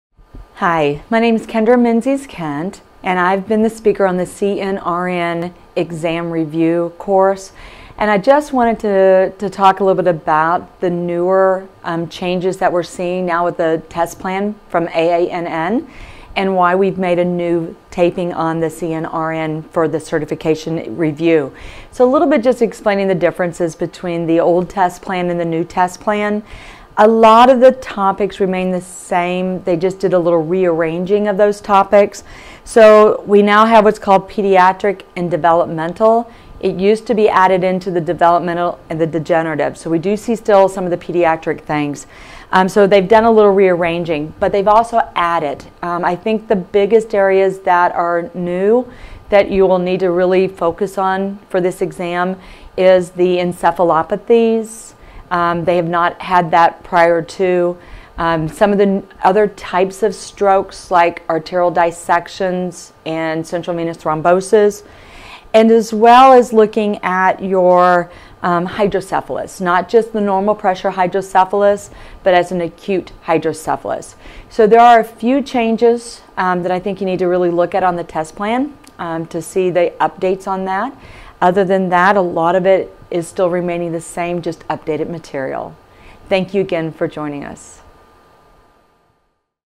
CNRN® Exam Review by Kendra Menzies Kent, MS, RN-BC, CCRN, CNRN, SCRN | Promo - MED-ED
This is a promo video on our CNRN Exam Review eLearning course presented by Kendra Menzies Kent, MS, RN-BC, CCRN, CNRN, SCRN.

The CNRN Exam Review course is designed to provide the neuroscience nurse with a review of the topics presented in the CNRN Examination. The course outline is based upon the blueprint of the CNRN Examination provided by ABNN. The course provides the participant with hints and easy ways to remember certain pertinent information.